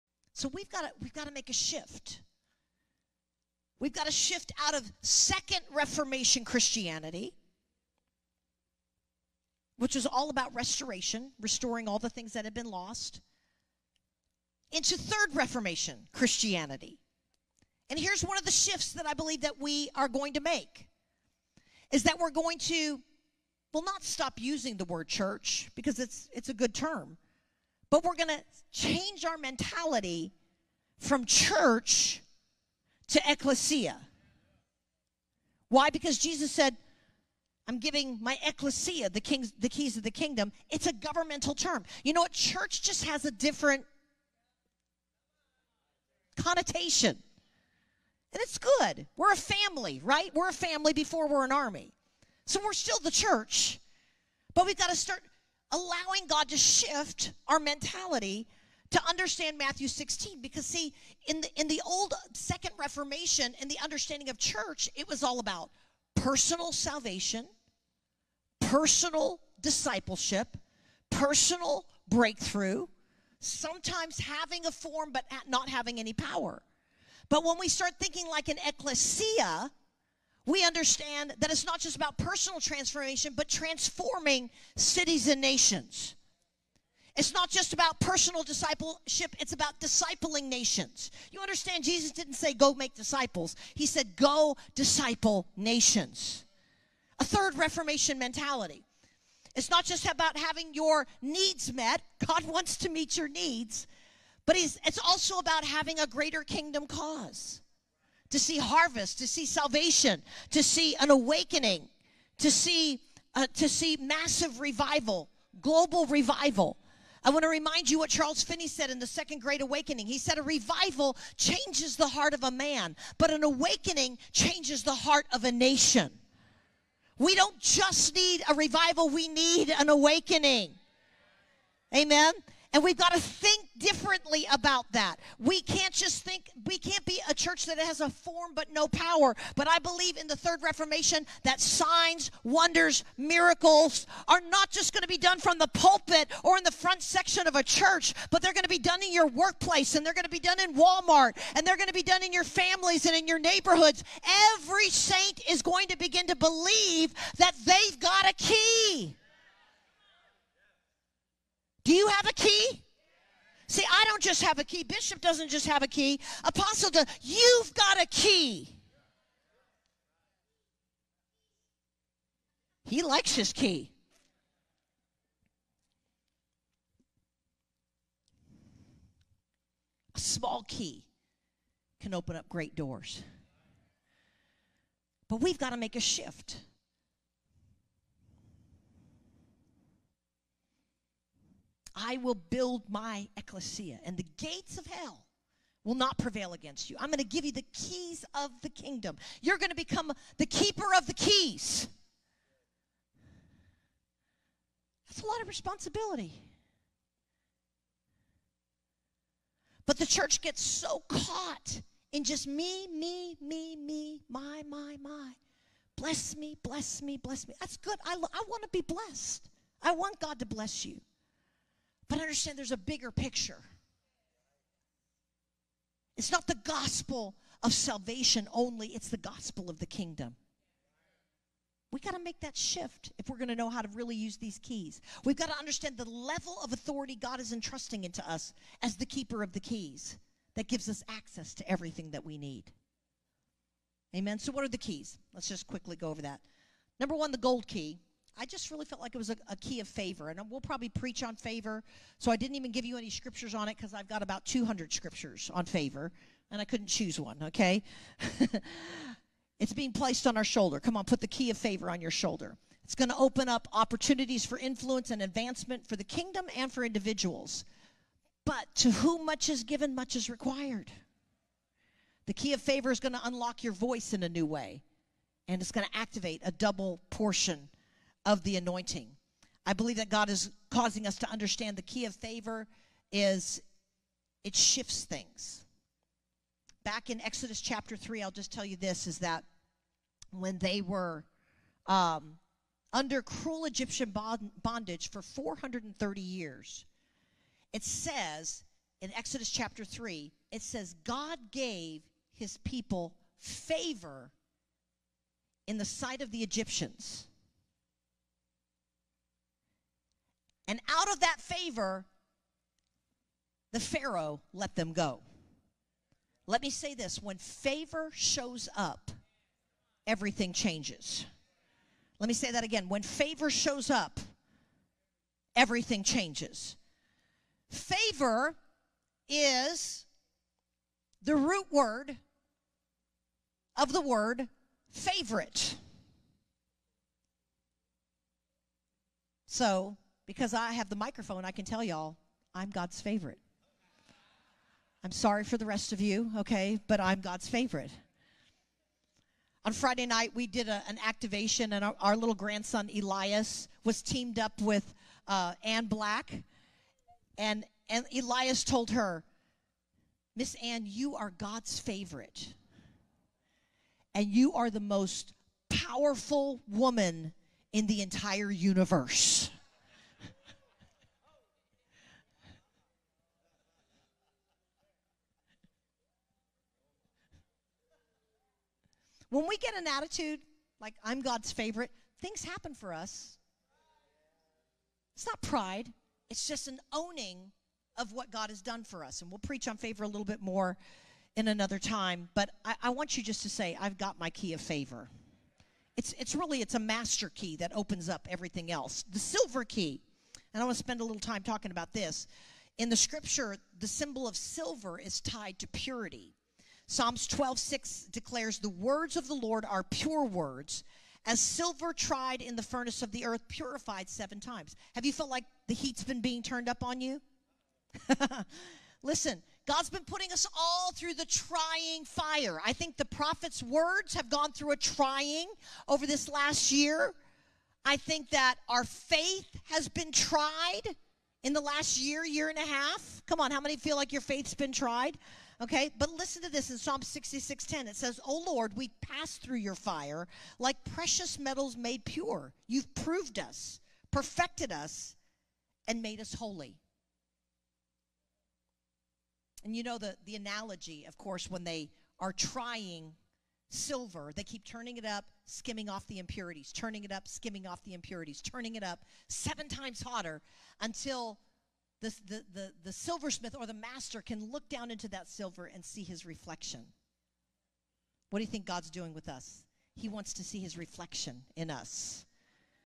Jane Hamon: Small Keys Open Big Doors (Isaiah 22:22)
Jane Hamon: Small Keys Open Big Doors (Isaiah 22:22)
Jane speaks at Vision Church at Christian International on January 2, 2022. Isaiah 22:22 says, “The key of the house of David I will lay on his shoulder; so he shall open, and no one shall shut; and he shall shut, and no one shall open.” Quotes: “To whom much is given much is required.” The key of favor is going to unlock your voice in a new way. And it's going to activate a double portion of the anointing. I believe that God is causing us to understand the key of favor is... it shifts things. Back in Exodus 3 - I'll just tell you this is that when they were under cruel Egyptian bondage for 430 years, it says in Exodus 3, "God gave his people favor in the sight of the Egyptians.”

Be blessed.

We've got to make a shift. We've got to shift out of second reformation Christianity, which is all about restoration, restoring all the things that have been lost, into third reformation Christianity. Here is one of the shifts that I believe that we are going to make - we're going to... we'll not stop using the word church because it's, it's a good term.

But we're going to change our mentality from church to ekklesia. Why? Because Jesus said I'm giving my ekklesia, the keys of the kingdom - It's a governmental term. You know what? Church just has a different connotation and it's good. We're a family, right? We're a family before we're an army. So we're still the church, but we've got to start allowing God to shift our mentality, to understand Matthew 16... because see, in the, in the old second reformation and the understanding of church, it was all about personal salvation, personal discipleship, personal breakthrough.

Sometimes having a form... but not having any power. But when we start thinking like an ekklesia, we understand that it's not just about personal transformation but transforming cities and nations. It's not just about personal discipleship. It's about discipling nations. You understand Jesus didn't say go make disciples. He said go disciple nations. A third reformation mentality. It's not just about having your needs met. God wants to meet your needs. But it's also about having a greater kingdom cause. To see harvest, to see salvation, to see an awakening, to see, to see massive revival, global revival.

I want to remind you what Charles Finney said in the second great awakening. He said, “A revival changes the heart of a man, but an awakening changes the heart of a nation. We don't just need a revival; we need an awakening. We've got to think differently about that. We can't just think, we can't be a church that has a form, but no power. But I believe in the third reformation, that signs, wonders, miracles are not just going to be done from the pulpit, or in the front section of a church, but they're going to be done at your workplace and they're going to be done in Walmart, and they're going to be done in your families and in your neighborhoods.

Every Saint is going to begin to believe that they've got a key. Do you have a key? A small key can open up great doors, but we've got to make a shift. I will build my ekklesia and the gates of hell will not prevail against you. I'm going to give you the keys of the kingdom. You're going to become the keeper of the keys. That's a lot of responsibility. But the church gets so caught in just me, me, me, me, my, my, my, bless me, bless me, bless me. That's good. I want to be blessed. I want God to bless you.

But understand there's a bigger picture. It's not the gospel of salvation only. It's the gospel of the kingdom. We got to make that shift if we're going to know how to really use these keys. we've got to understand the level of authority God is entrusting to us as the keeper of the keys that gives us access to everything that we need. So what are the keys? Let's just quickly go over that. Number one, the gold key. I just really felt like it was a key of favor and we will probably preach on favor.

So I didn't even give you any scriptures on it because I've got about 200 scriptures on favor and I couldn't choose one. It's being placed on our shoulder. Come on, put the key of favor on your shoulder. It's going to open up opportunities for influence and advancement for the kingdom and for individuals.

But to whom much is given much is required. The key of favor is going to unlock your voice in a new way. And it's going to activate a double portion of the anointing. I believe that God is causing us to understand the key of favor is... it shifts things. It says in Exodus 3, "God gave his people favor in the sight of the Egyptians.